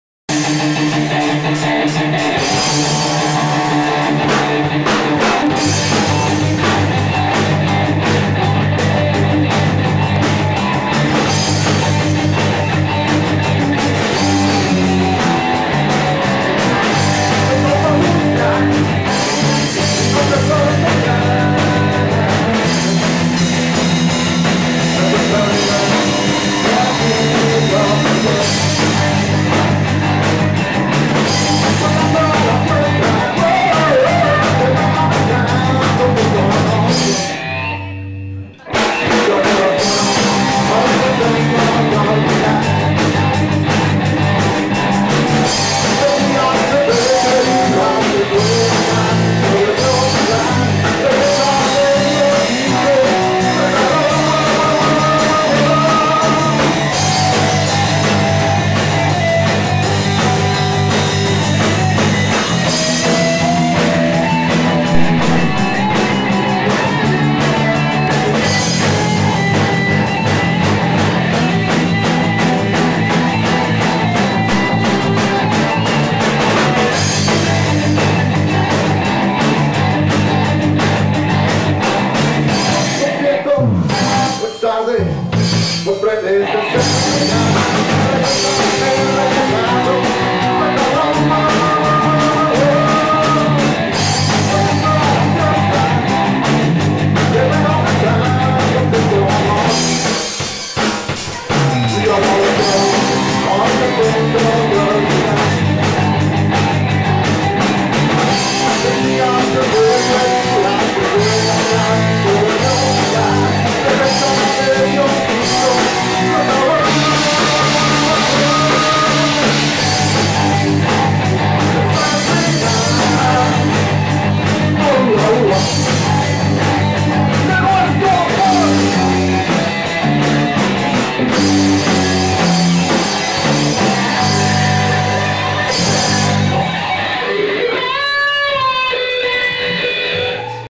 Palomazo con Leo
El joven leo en la bateria... tocando una cancion.. muy comun de bar.. aunk bueh!... no suena de lo mejor..para ser solo un Palomazo no esta mal... jejejeje